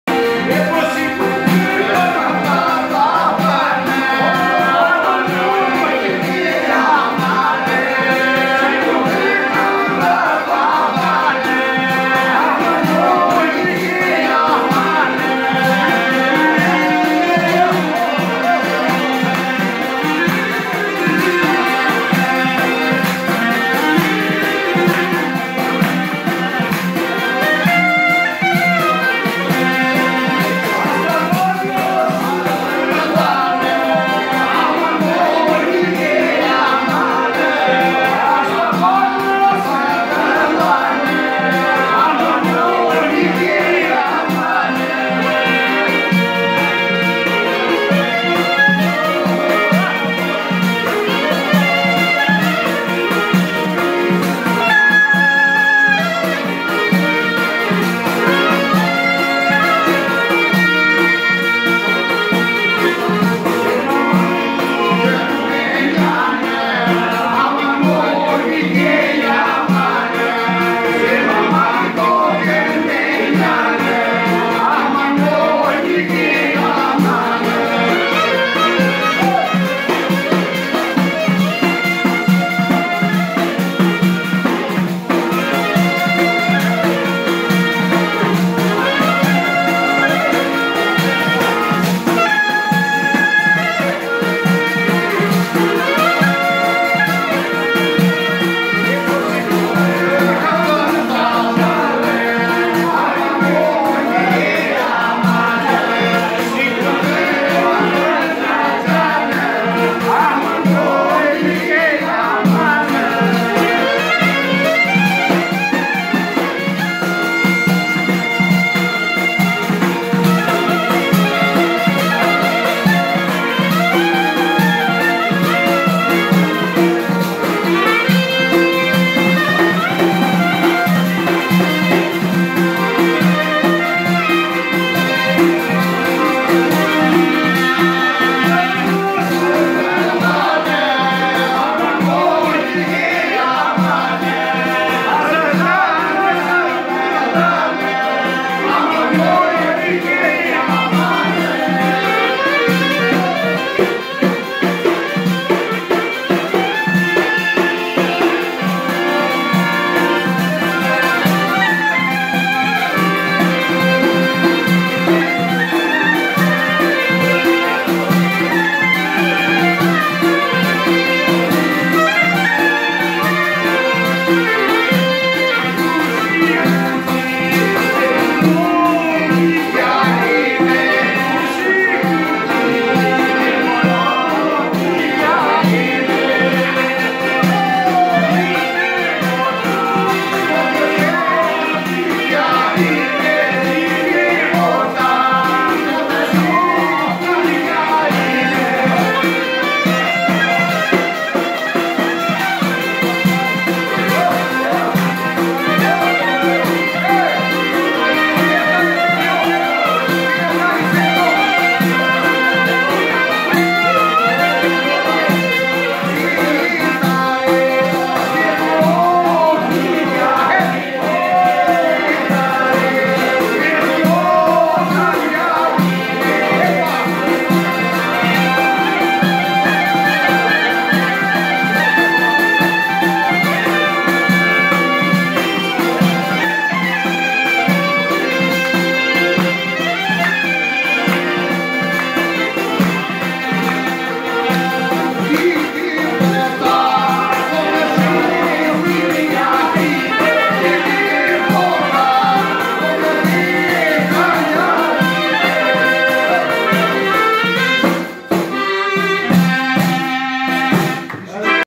Grupi Sazeve Myzeqeja & Pali Lule - ne Taverna Kristal Kemishtaj 2019
Tom Bali Kemishtaj